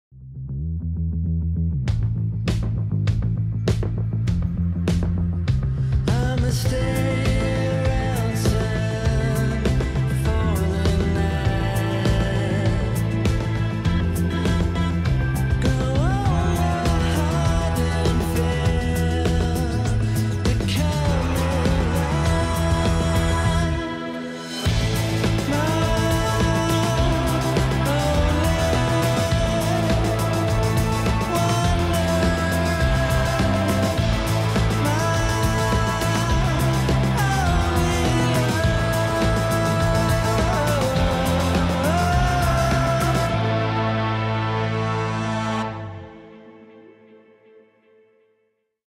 La nouvelle Peugeot 508
Voici la vidéo officielle de la nouvelle Peugeot 508 dans ses déclinaisons : berline, break et crossback.